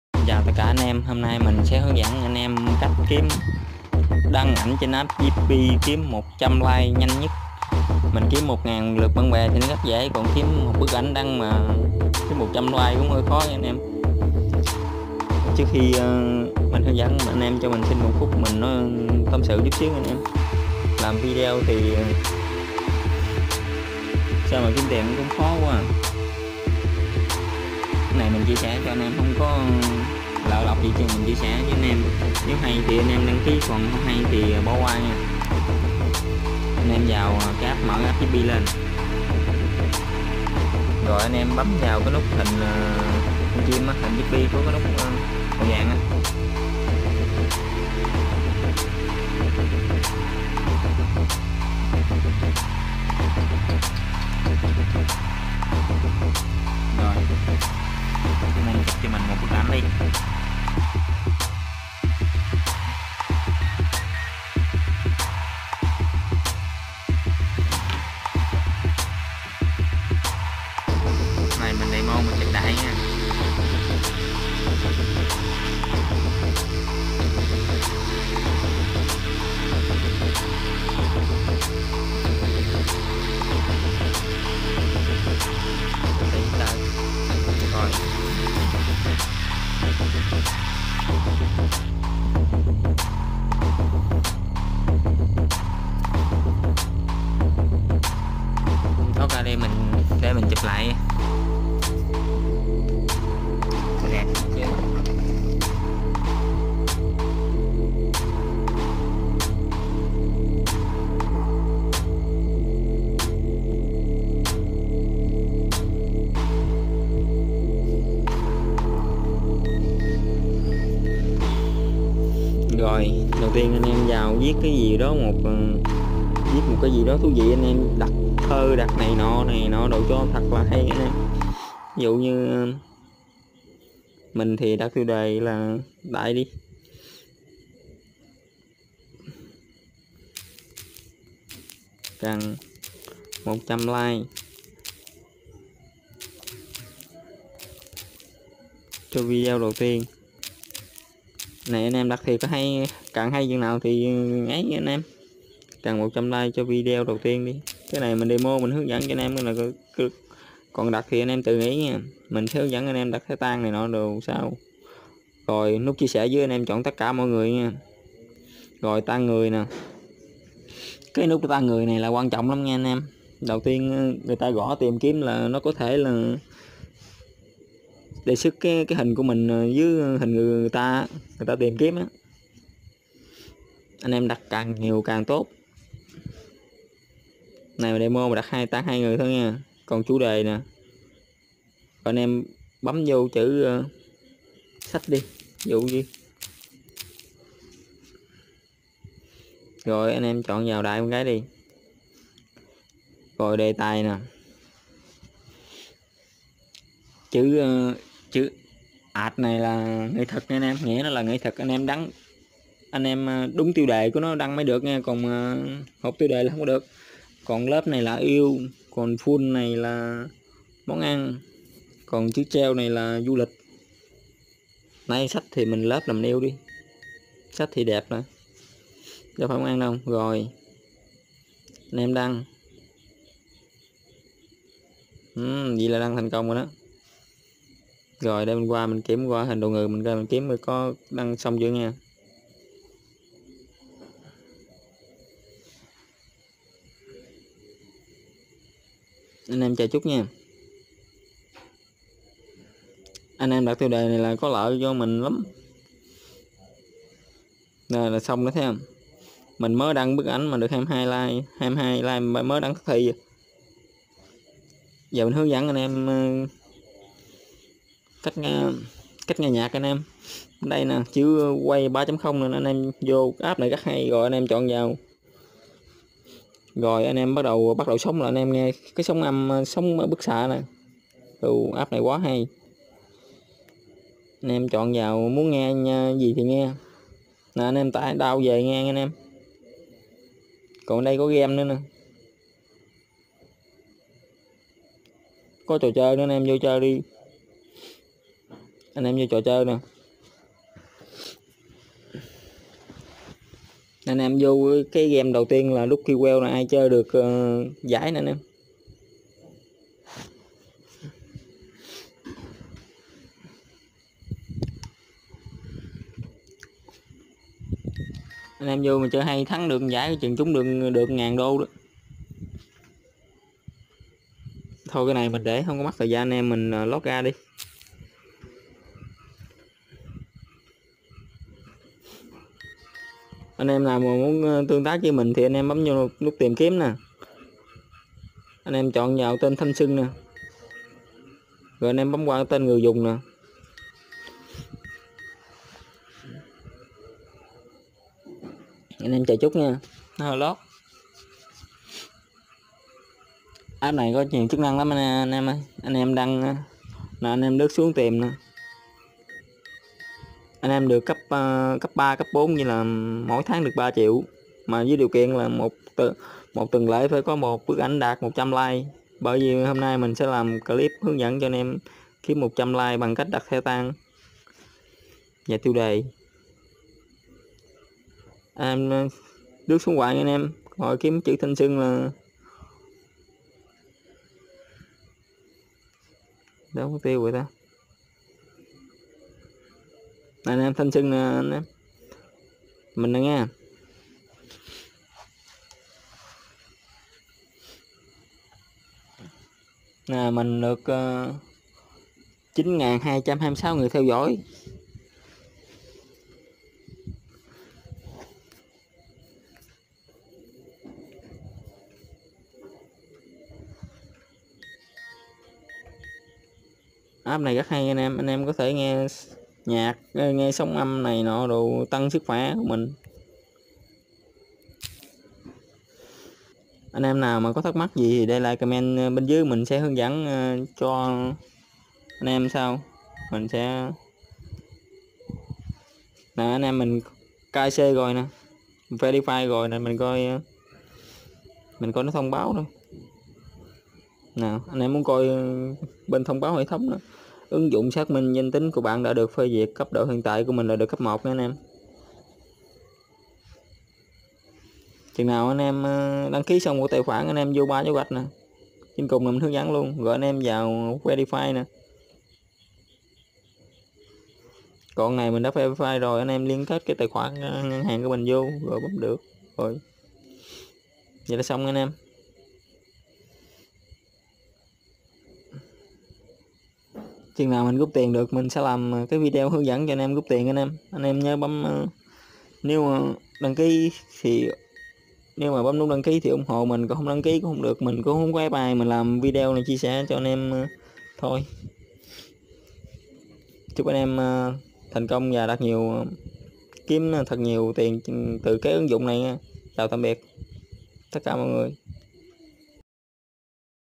đúng như lời đã hứa - hướng dẫn đăng ảnh yippi 100like một ngày không khó
Link hướng dẫn ĐK áp yippi

Link tải áp

Yippi is a messaging app with a focus on entertainment and security, It’s fast, simple, secure and free